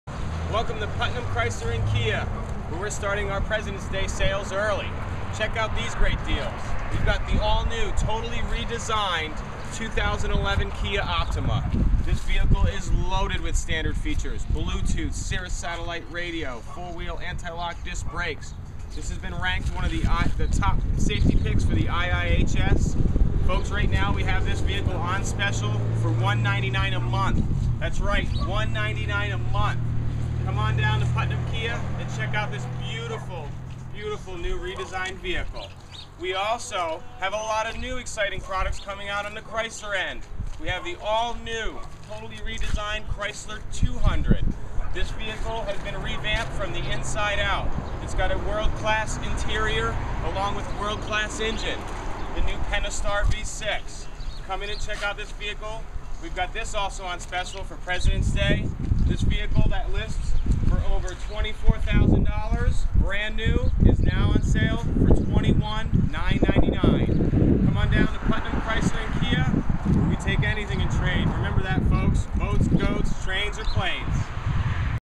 Chrysler & Kia highlighted at Putnam Chrysler Dodge Jeep Kia in Putnam, CT.
Jason Duquet give you two great reasons to stop by Putnam Chrysler Dodge Jeep Kia in Putnam, CT. Check out the all new 2011 Kia Optima and the 2011 Chrysler 200!!